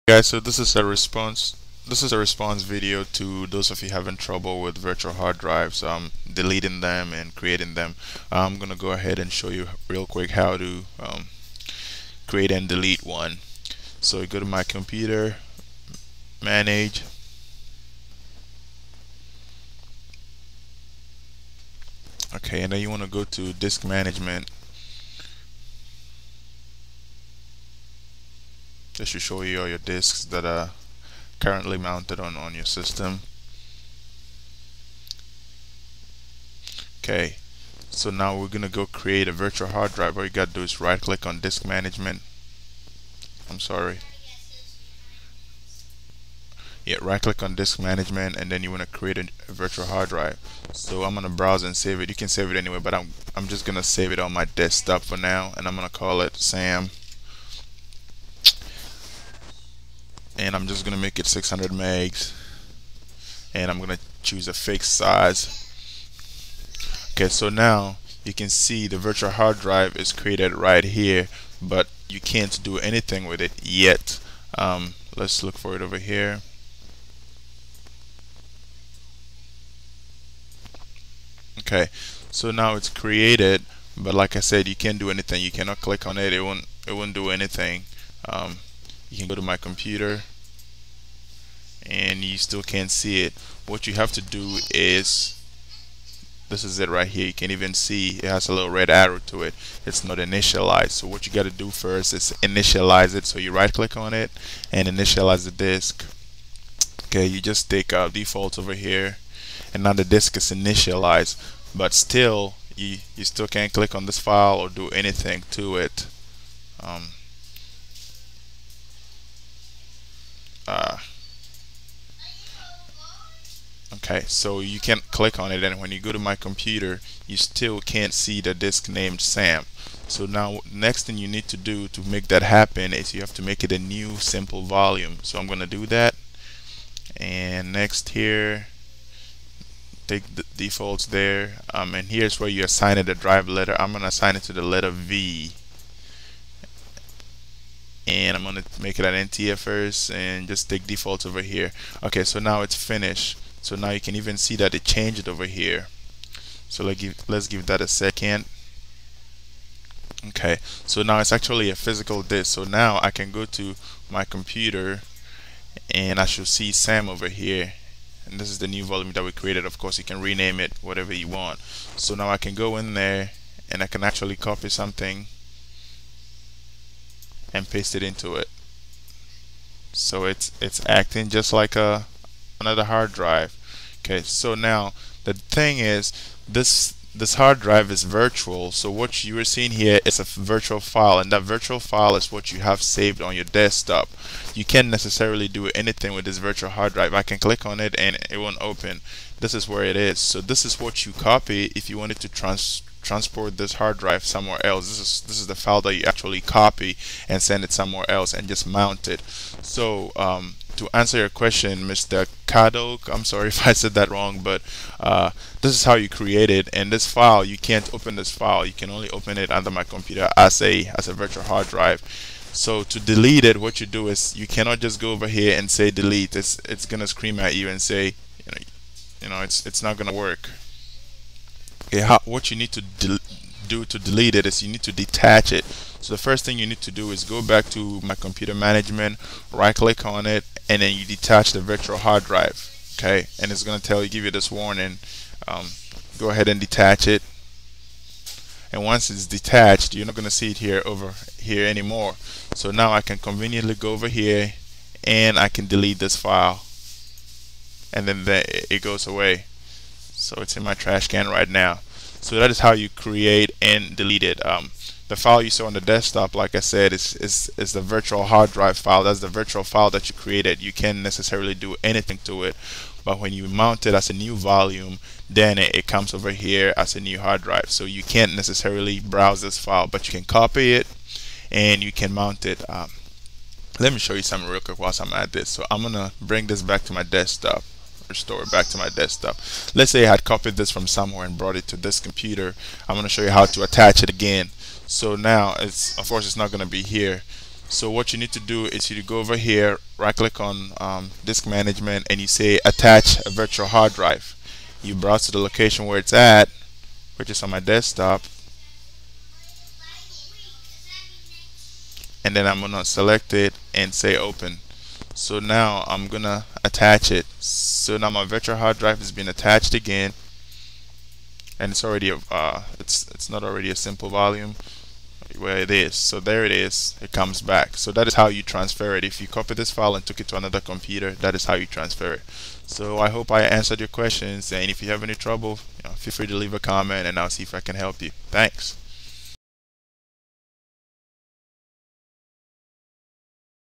Create and Delete VHD in Windows 7 Response Video .avi
This is a response to  How to create  a VHD ."This video shows how to create , delete and mount a copied VHD